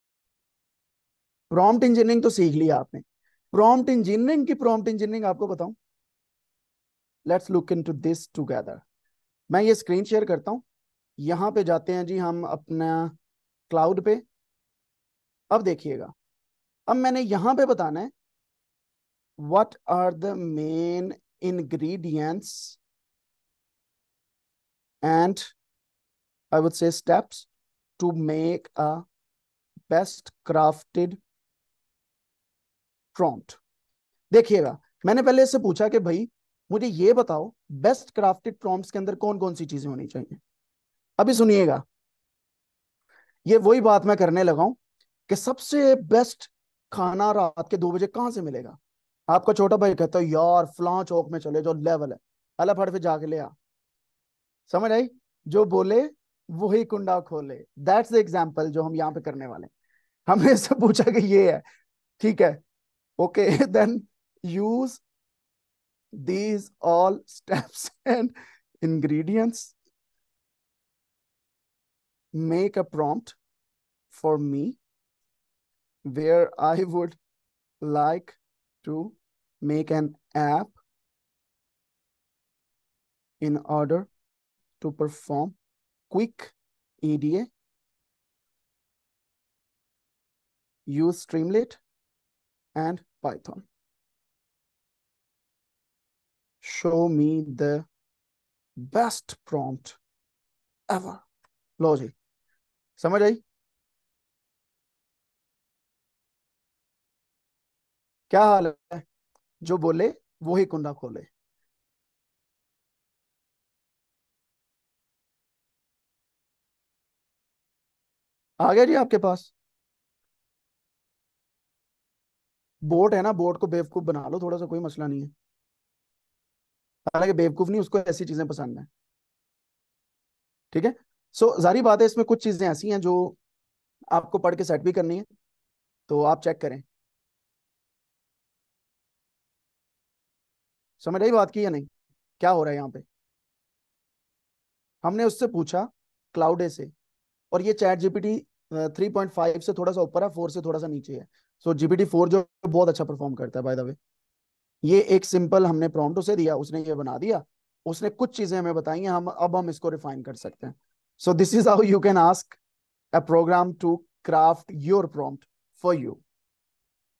Using Prompt Engineering to Craft Powerful Prompts with AI
Our Current Live Course:
AI Ka Chilla 2023
To register for AI ka chilla 2023, fill this google form

Video Description:
In this video, I'll show you how to do prompt engineering by using AI assistants to help craft prompts. I'll demonstrate techniques like iterative self-dialogue, searching for relevant contexts, and generating template prompts that you can customize. By leveraging the knowledge and language skills of AI systems, you can create highly effective prompts to extract valuable insights, demonstrate new concepts, and even steer conversations in a desired direction. I'll also discuss best practices for prompt engineering safely and responsibly.